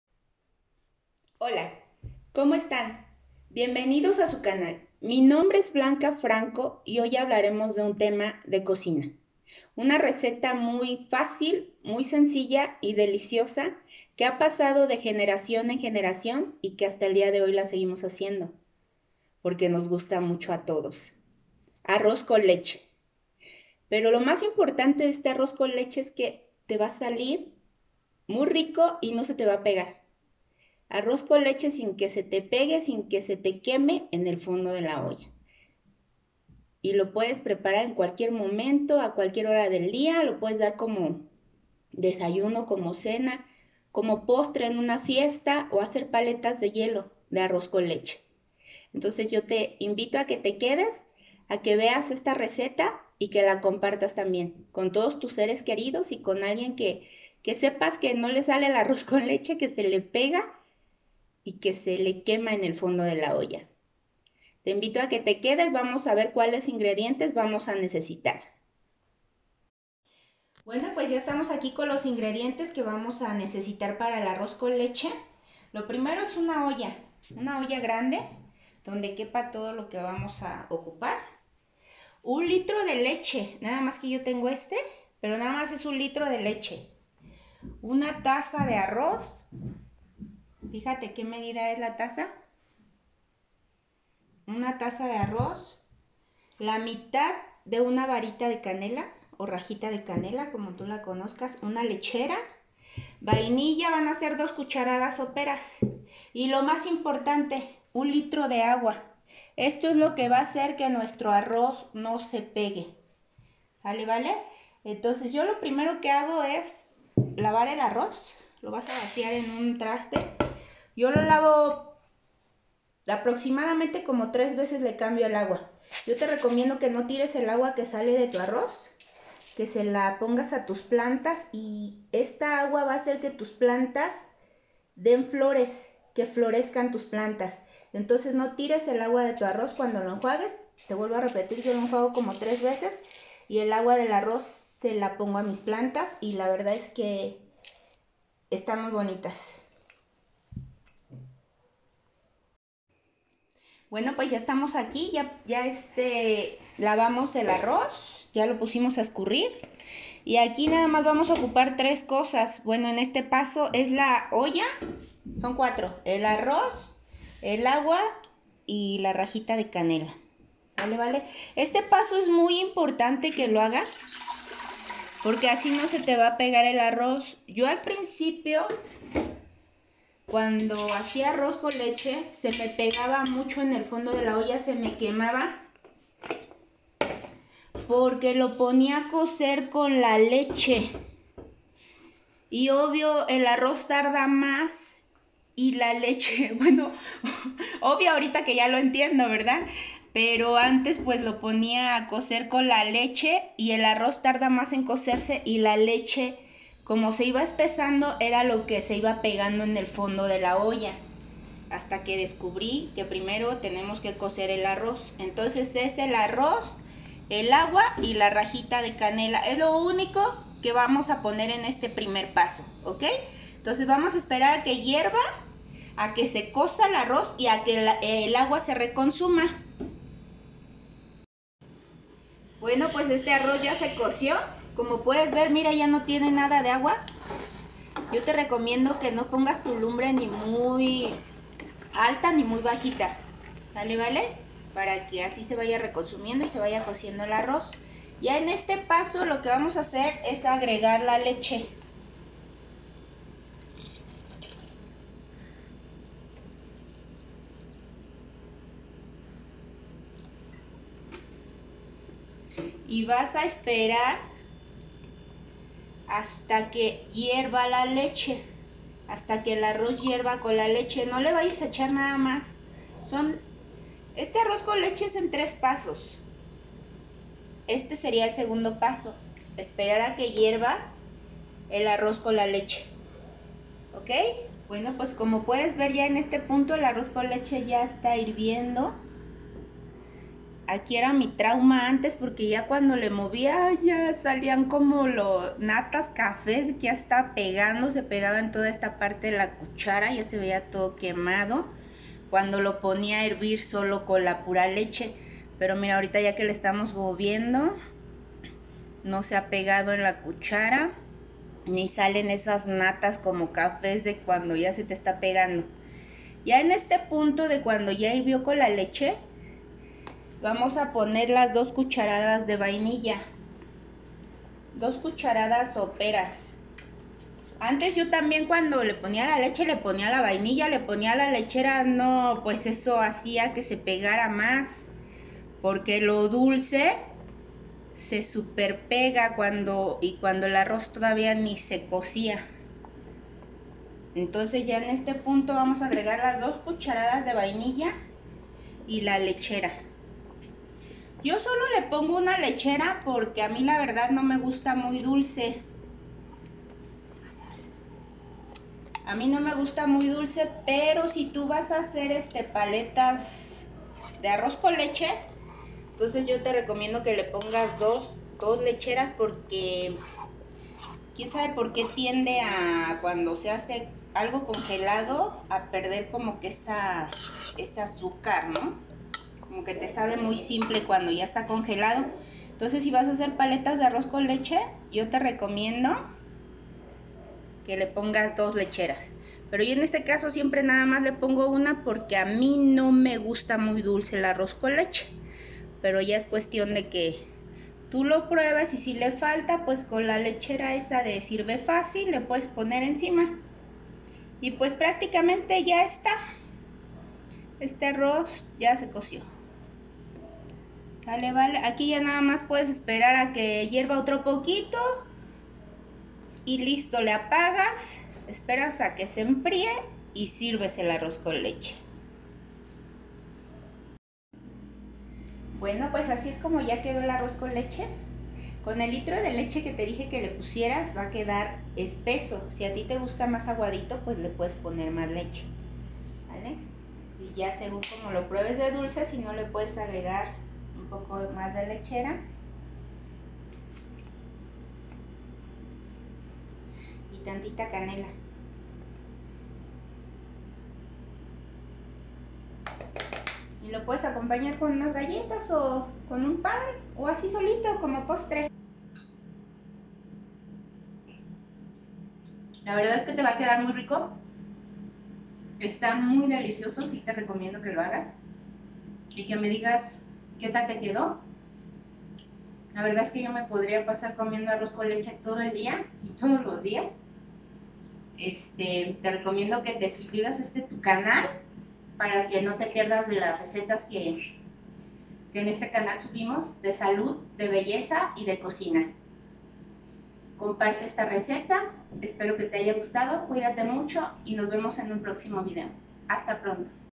Arroz con Leche, Perfecto👌y Delicioso!!!😋
COMO HACER UN ARROZ CON LECHE PERFECTO.
Nunca mas se te quemara ni se volvera a pegar  tu arroz con leche.
Asi de seguros estamos y si no te devolbemos tu dinero; a no verdad no aplica pero te lo firmo que te va a quedar un arroz con leche perfecto y delicioso si sigues los pasos de la receta al pie de la letra.

INGREDIENTES:
Una olla grande.
1 litro de agua.
1 litro de leche.
2 cucharadas soperas de vainilla.
1/2 rajita de canela.
1 lata de lechera.
1 taza de arroz.
Y manos a la olla.